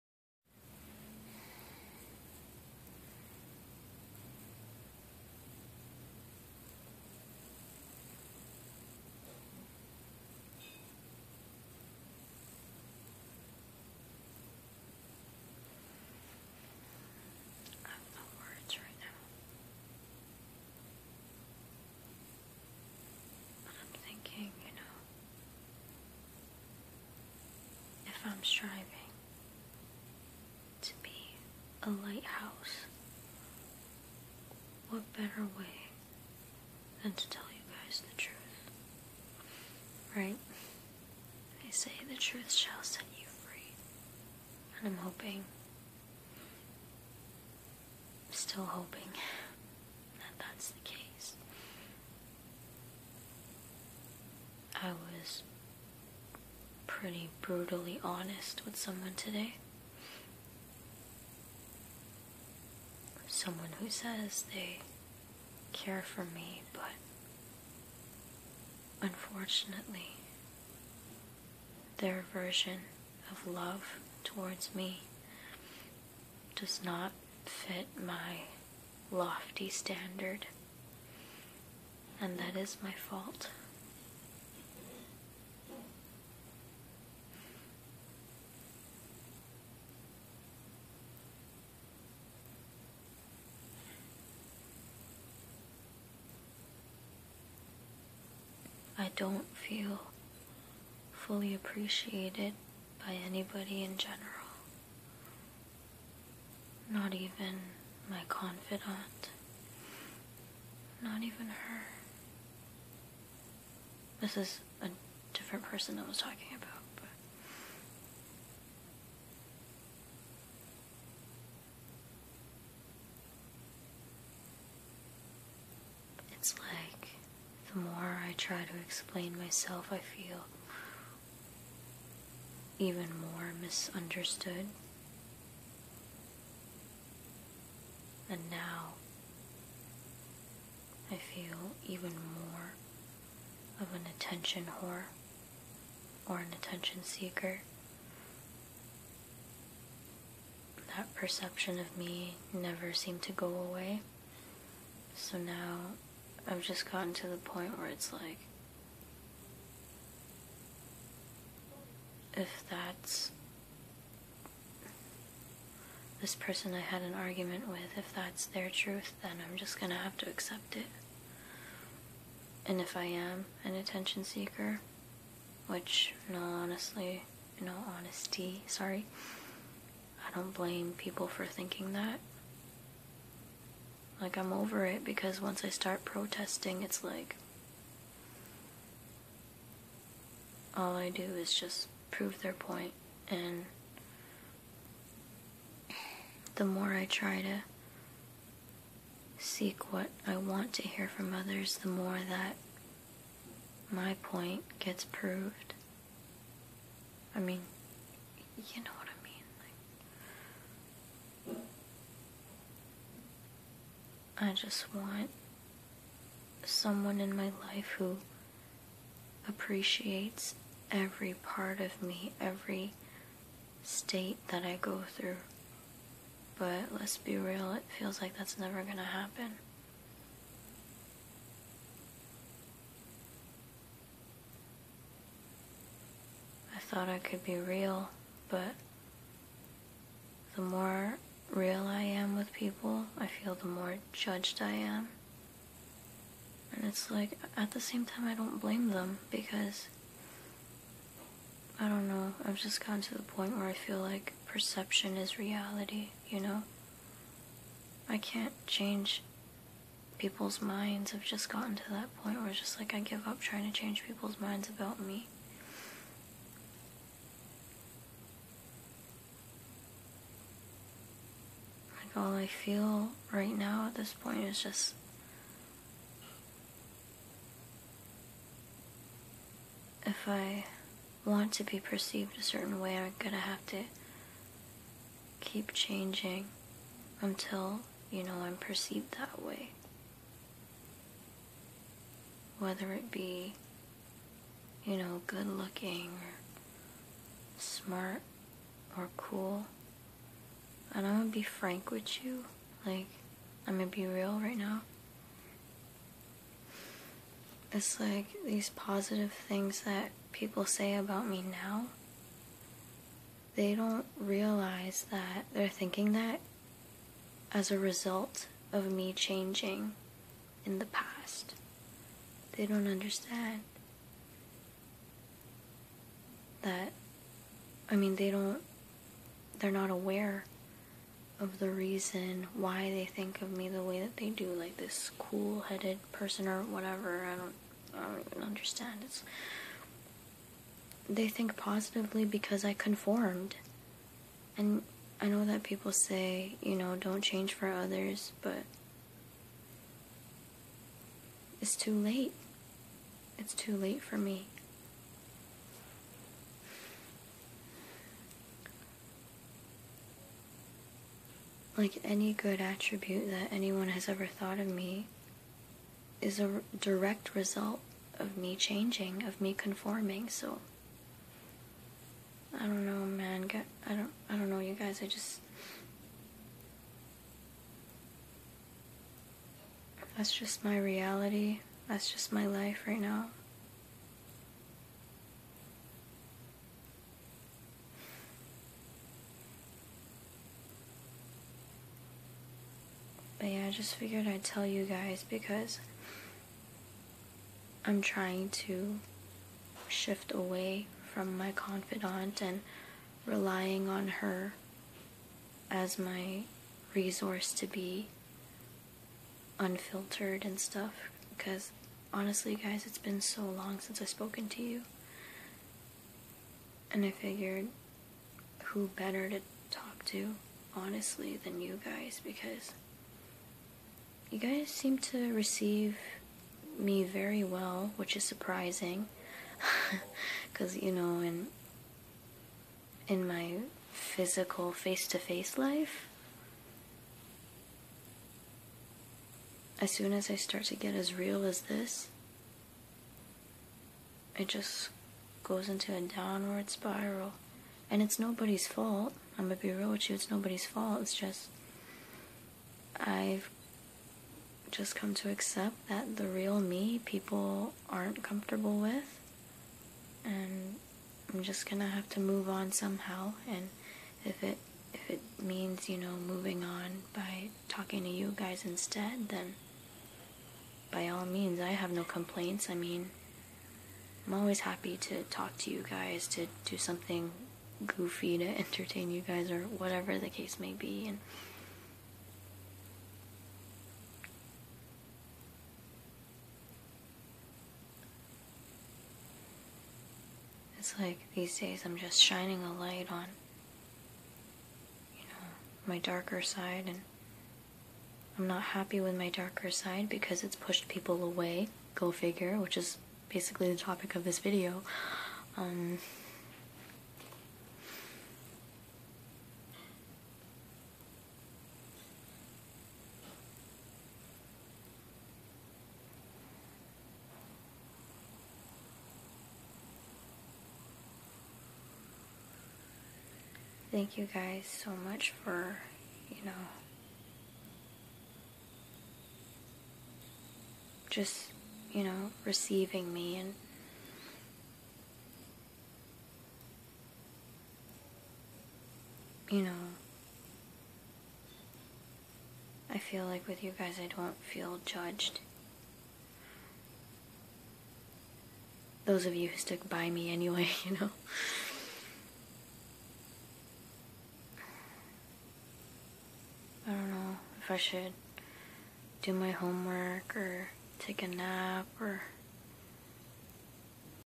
*2017 revisit* They say don't change for others...
Forgive my vagueness, guys. I didn't feel like crying on camera. Basically I had an argument with someone relatively close to me in my life (whose identity I'd rather keep private) about how attractive people are more privileged (did I spell that wrong?) than those who aren't considered attractive. It hurt me when I was told that wanting to look good for attention was wrong. It also hurt when I was told I don't understand how it feels to be liked only for my looks (as if I don't already feel unattractive enough). To be real, after thinking about it, I wish this person had said to me "You're beautiful in every way imaginable and anyone who can't see that is mad crazy!" Yes, my standards are ridiculously high when it comes to feeling appreciated. It's just that when this person carried on about their problem of being treated a certain way solely based on their good looks, instead of empathizing with me, I felt judged and misunderstood. Eventually I felt invalidated by this person when they told me I feel rejected because I push others (specifically them) away. The more this person reiterated to me that good looking people get taken advantage of and how I shouldn't want that, the more unattractive I felt. Their version of help just wasn't helpful for me personally. It felt like instead of helping me overcome my lack of confidence, this person seemed like they were trying to prove their problems are worse than mine. Also, I brought up that part of why I felt so unattractive was that this person told me "no one wanted me". They didn't apologize but instead they got mad that I "held it against them for years". I wish I explained that I couldn't just trust them immediately and open up to them after that, but I knew the argument was going nowhere so I gave up.